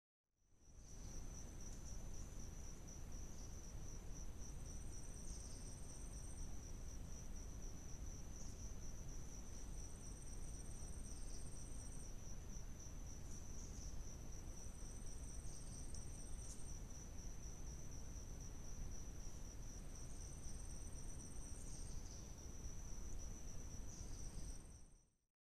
Okapi Peering Around Meadow
The Okapi Conservation Project is located within the Ituri Forest, in the Democratic Republic of Congo. The Okapi Wildlife Reserve was created in 1992, encompassing 13,700 square kilometers, in 1996, it was designated as a United Nation's World Heritage Site.